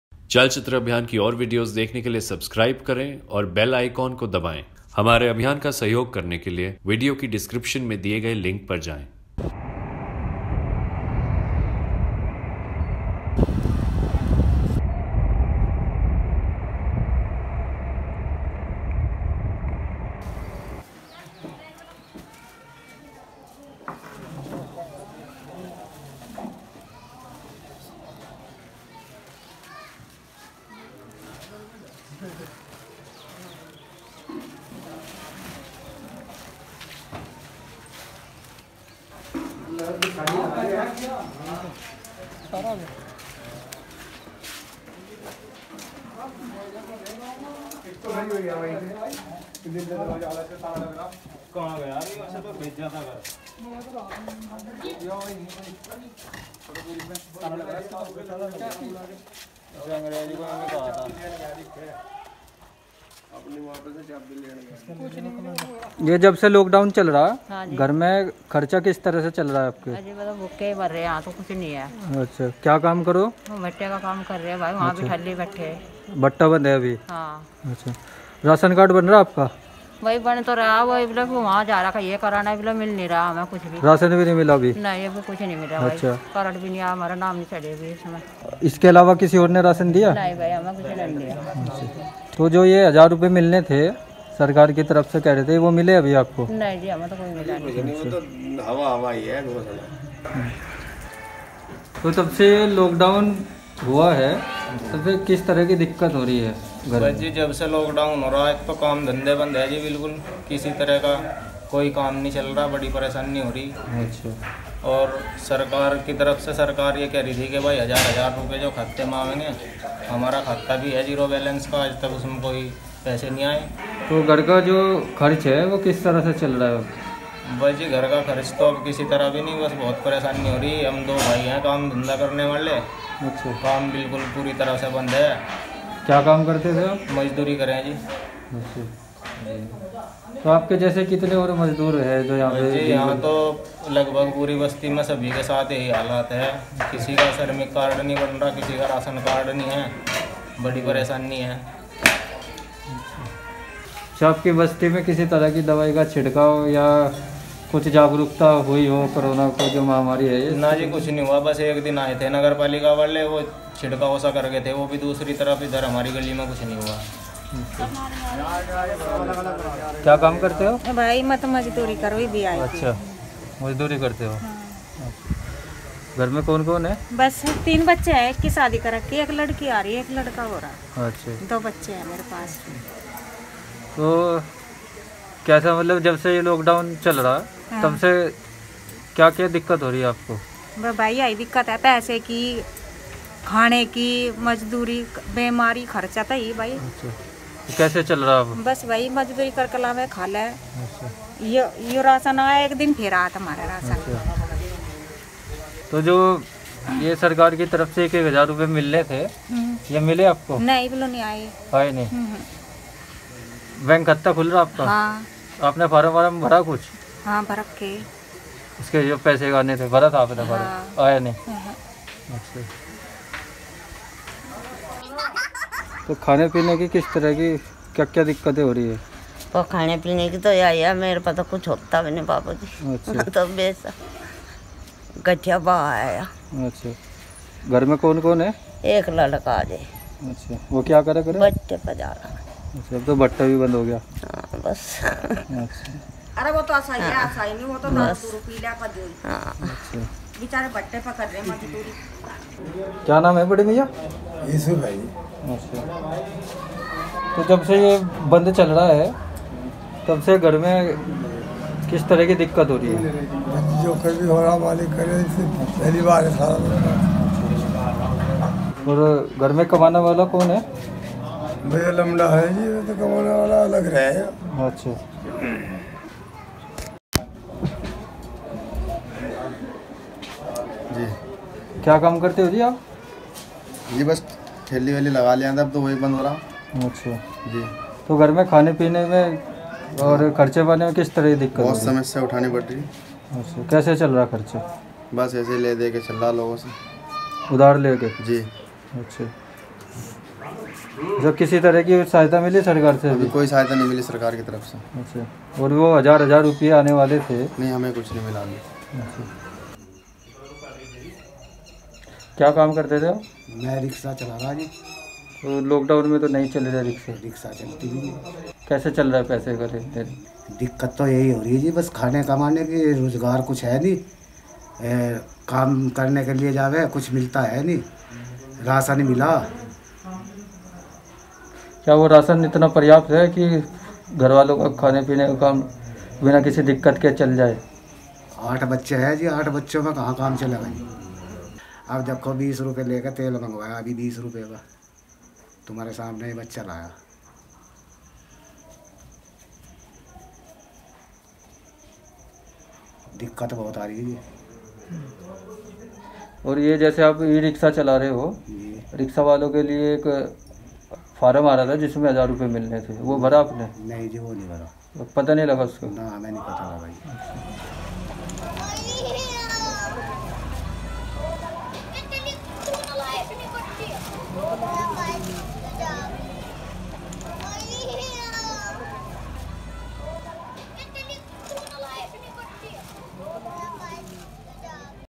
लॉक डाउन में अभी भी नहीं मिल रहा मज़दूरों को राशन l Workers Not Getting Ration During Lock Down
उत्तर प्रदेश के शामली ज़िले के काँधला क़सबे के कई मज़दूरों का आरोप है कि उन्हें अभी तक सरकार की तरफ़ से, लॉक डाउन में, अभी तक न तो राशन मिला है न कोई और मदद मिल रही है। वह मुख्य रूप से स्थानीय समाजिक संगठनों की मदद से अपना गुज़ारा कर पा रहे हैं।
टीम : विशाल स्टोनवाल , शाकिब यामीन रंगरेज़ , नकुल सिंह साव्हने, राहुल शेरवाल
Workers in Uttar Pradesh's Kandhla township in Shamli district are alleging that several of them are still not receiving ration or any other help promised to them by the government. They are largely surviving by packets of ration given to them by charitable organisations in the area.
Team: Vishal Stonewall, Shakib Yameen Rangrez, Nakul Singh Sawhney and Rahul Sherwal